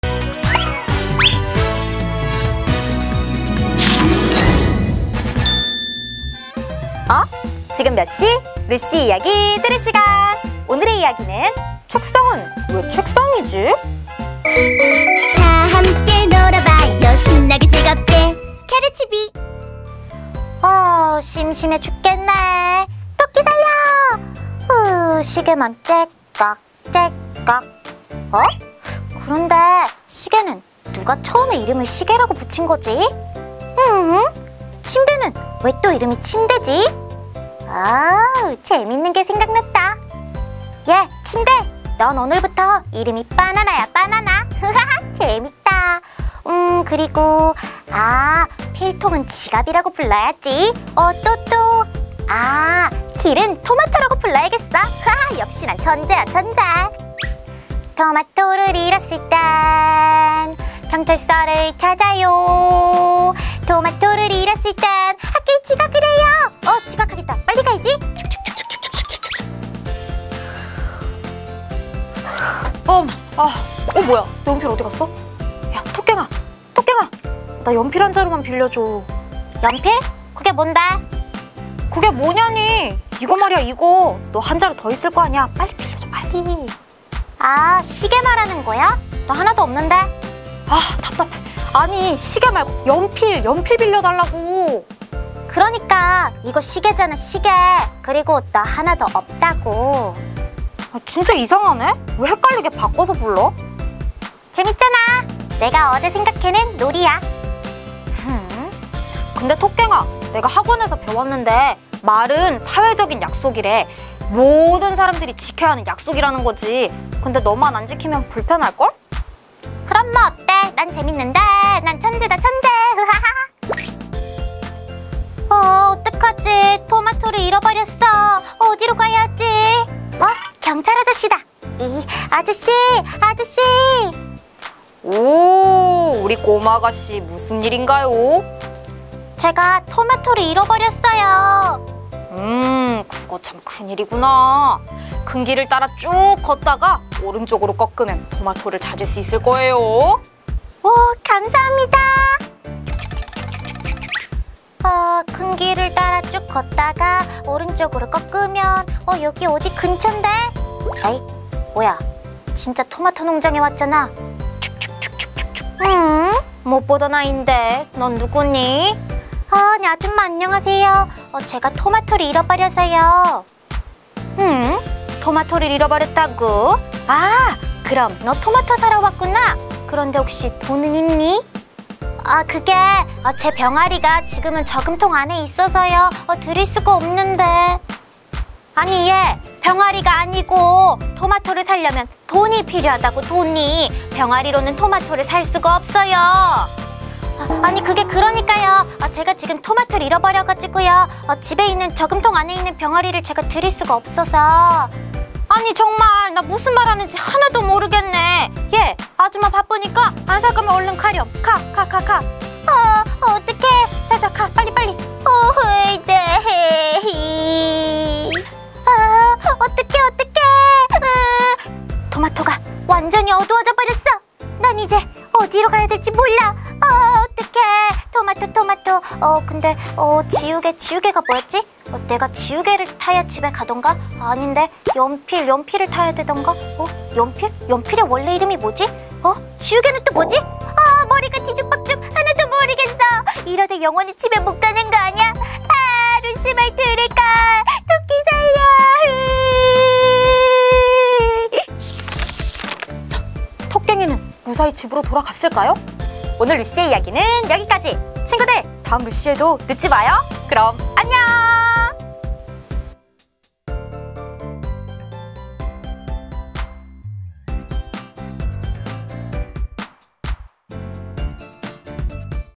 [생활동화] 책상은 왜 책상이지? | CarrieTV_Books
루시는 토깽이의 말이 이해가 안 돼요.
토깽이가 자꾸 연필을 시계라고 하거든요.
"말은 모든 사람이 지켜야 하는 사회적인 약속이라고!"
루시가 말했지만 토깽이는 루시를 무시해요.
어떤 것이든 이름을 바꿔 말하는 토깽이는 다른 사람들과 말이 통할까요?